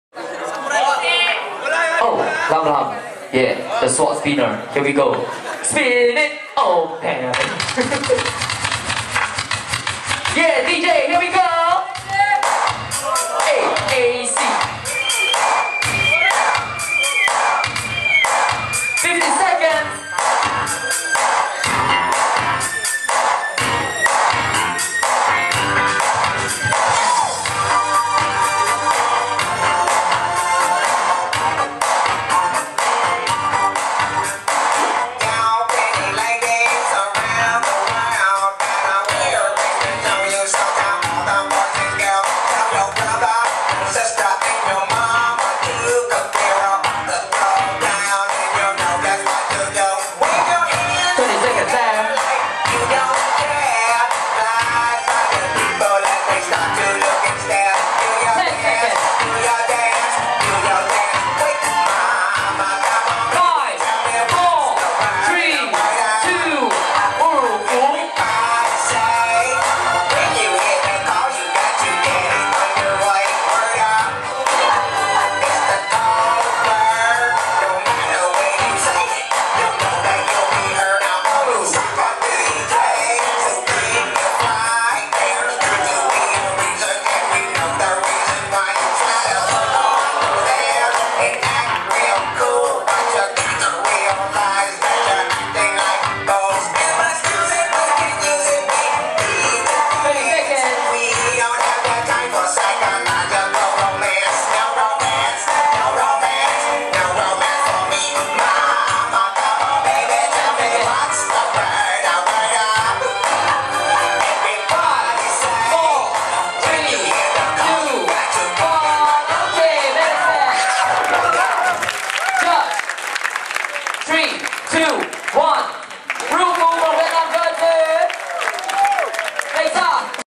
[King Of Hits Queen Of Beats 2011] Popping Top 16 - Battle 4
KING OF HITS QUEEN OF BEATS
Popping Top 16

Battle 4

7 August 2011
Republic Poly TRCC Dance Studio 1

Judges: Tyrone Proctor & PoppinJ
Organisers: Robin Hood & Cat Lady

Video recorded by Cepheus